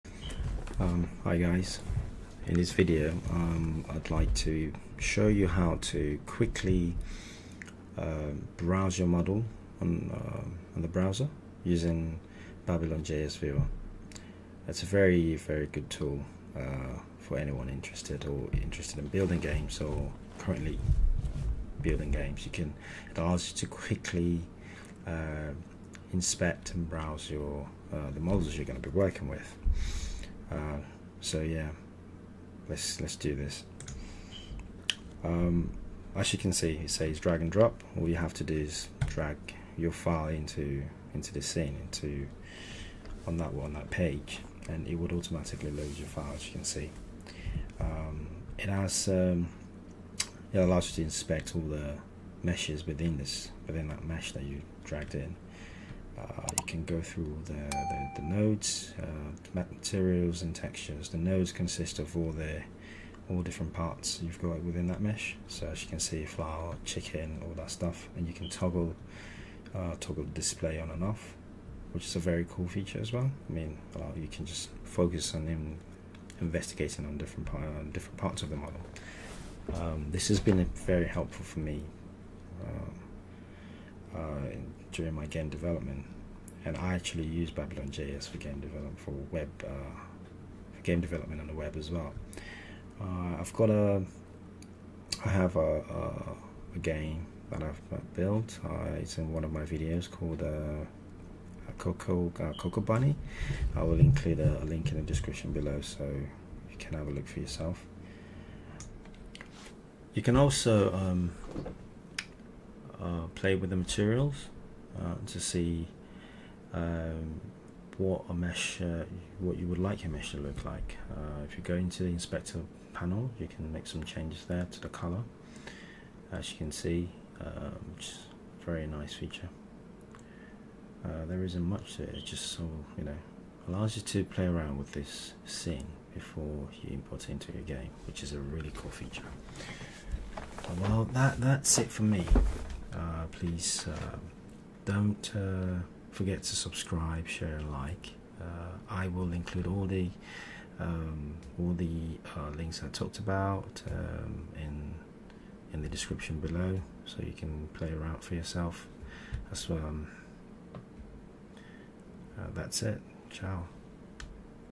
How to load Blender 3D models in BabylonJS viewer
See how to load blender 3d models in babylonjs online viewer. You can quickly and easily inspect a 3d model or scene before using it in your projects, completely online.